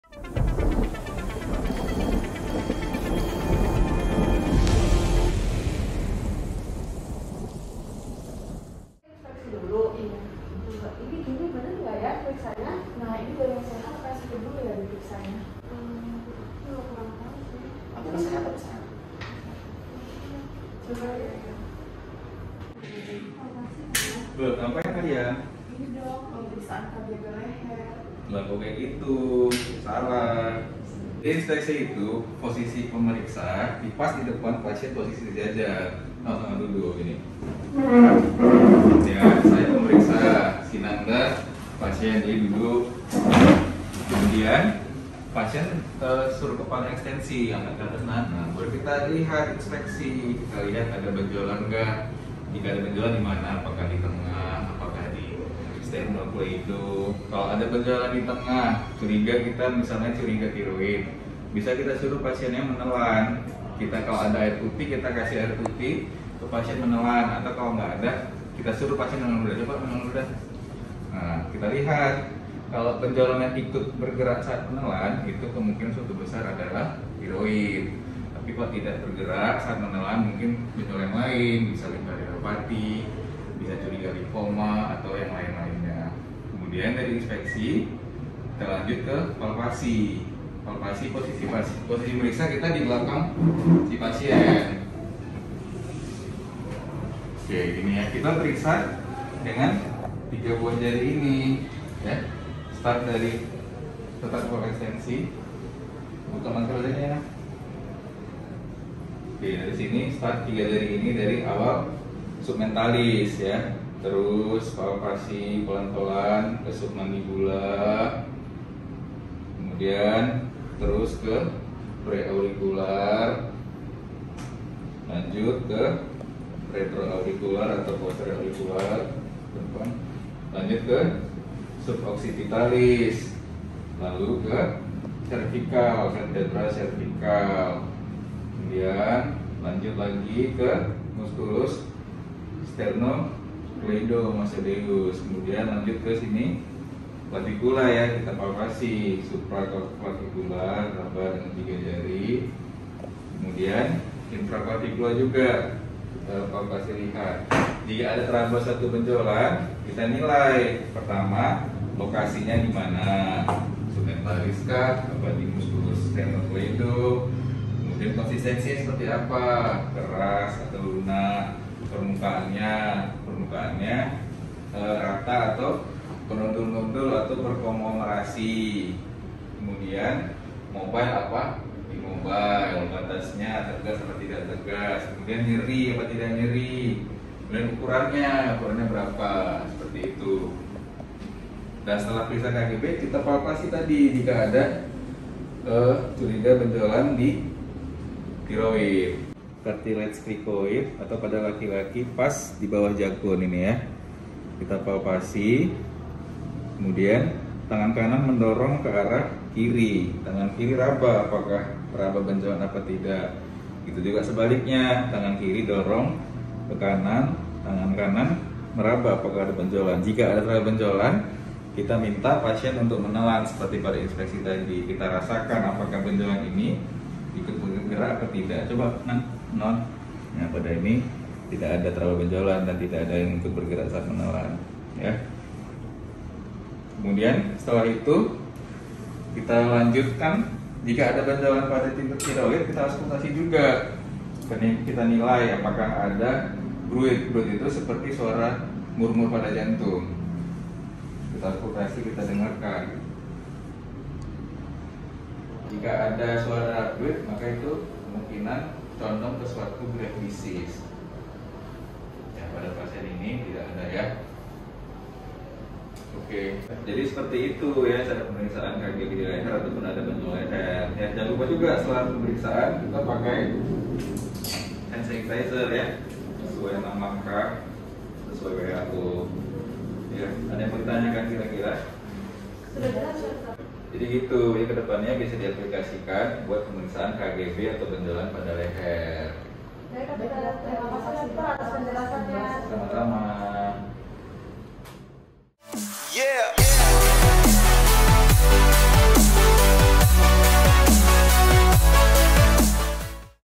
Pemeriksaan KGB Leher
Untuk dapat menegakkan diagnosis suatu kelainan atau penyakit pada leher diperlukan kemampuan dan keterampilan melakukan anamnesis dan pemeriksaan kgb leher. Kemampuan ini merupakan bagian dari pemeriksaan fisik yang merupakan syarat bila terdapat keluhan atau gejala yang berhubungan dengan leher.
Sistem aliran limfe leher sangat penting untuk dipelajari, karena hampir semua bentuk radang atau keganasan kepala dan leher akan terlihat dan bermanifestasi ke kelenjar limfe leher.
Dengan mengetahui lokasi pembesaran KGB maka kita dapat mengerahkan kepada lokasi kemungkinan terjadinya infeksi atau penyebab pembesaran KGB pada leher.